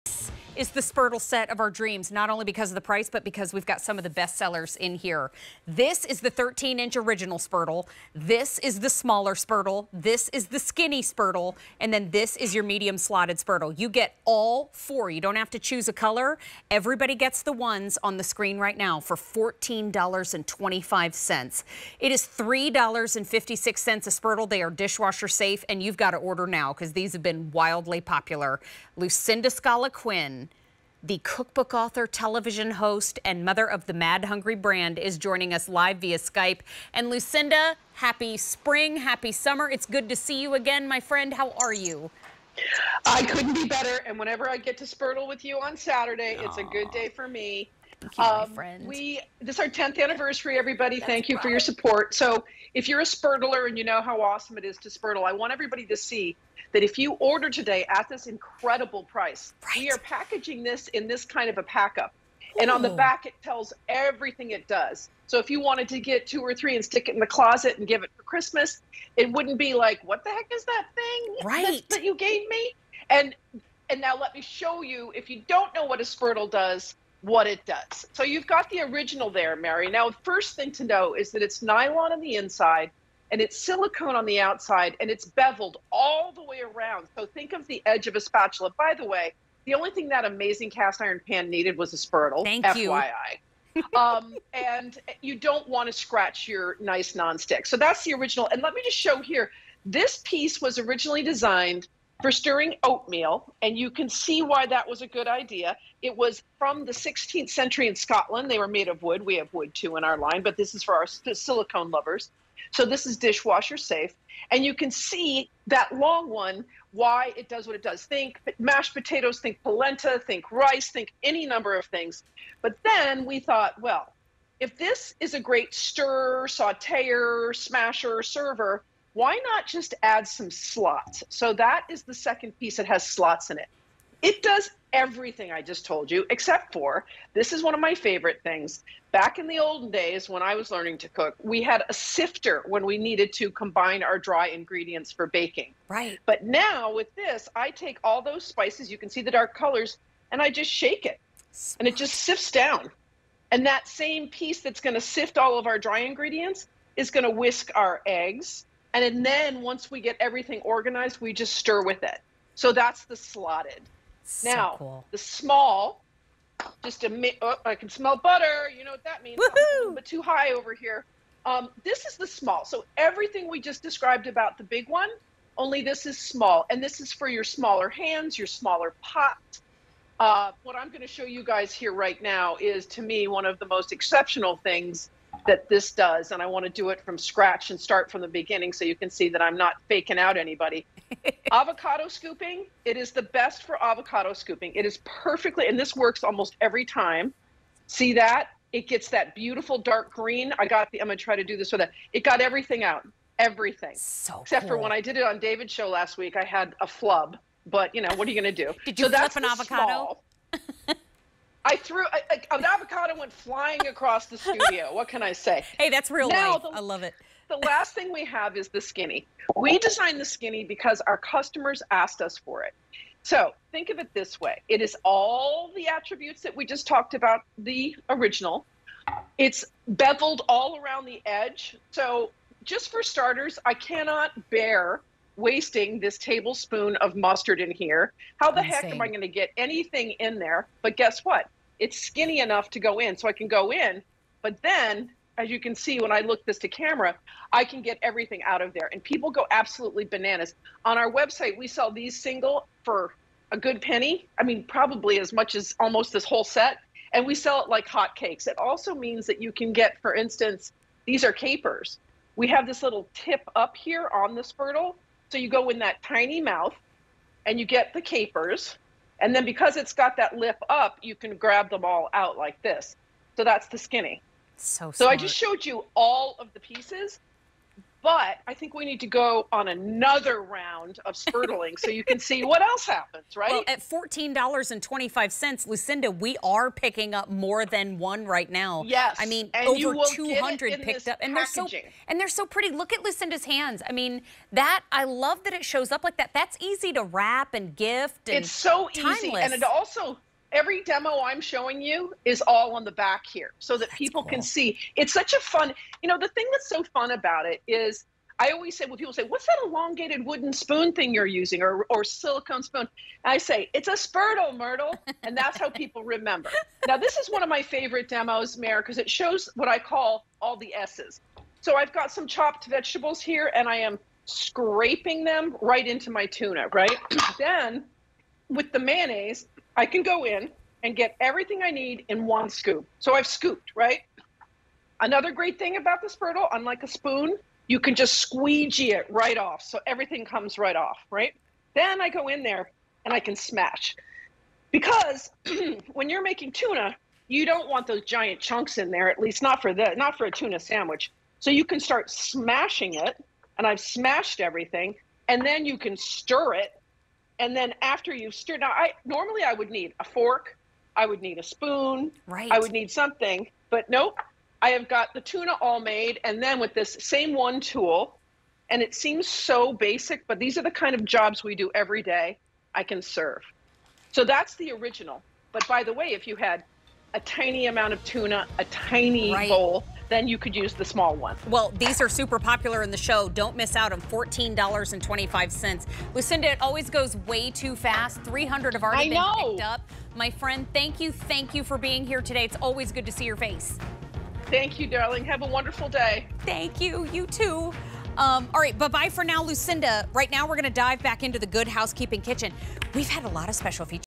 Mad Hungry 4-Piece Essential Silicone Spurtle Set on QVC
Mad Hungry 4-Piece Essential Silicone Spurtle Set

When every last morsel matters (and when doesn't it?), these silicone utensils let you stir, scoop, smash, sift, spread, scrape, and serve every bit of deliciousness. Their curved design gets into every last nook and cranny of your bowls, jars, pots, and pans, accessing edges and hard-to-reach areas with ease. From Mad Hungry.